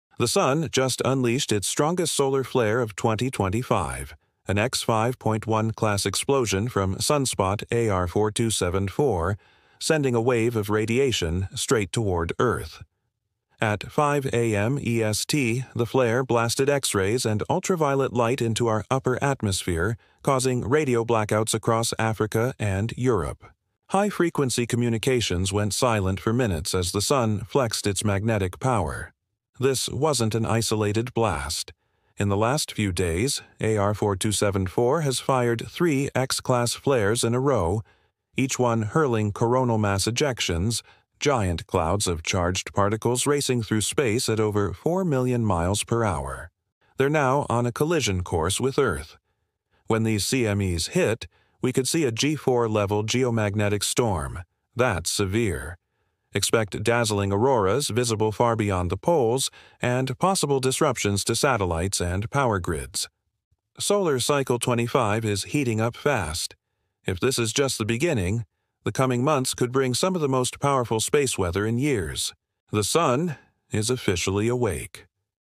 Massive solar flare event and its impact on Earth
“The Sun Just Exploded — X5.1 Flare Slams Earth!”

“Strongest Solar Flare of 2025 Hits Earth!”

“Radio Blackouts and Auroras: The Sun Strikes Again 🌞”

“Earth in the Crossfire: The Monster Flare from AR4274”

“Brace Yourself — The Sun’s Most Powerful Flare Yet!”